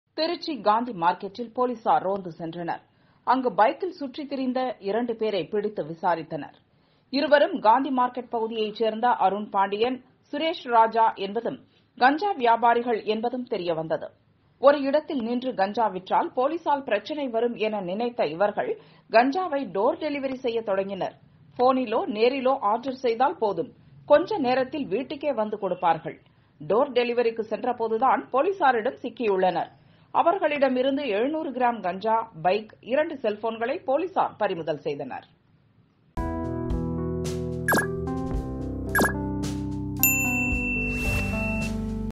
திருச்சியில் கஞ்சா டோர் டெலிவரி 2 ஆசாமிகள் கைது | Kanja | Tobacco
திருச்சியில் கஞ்சா டோர் டெலிவரி 2 ஆசாமிகள் கைது | Kanja | Tobacco#kanja tobacco dinamalarnews For more videos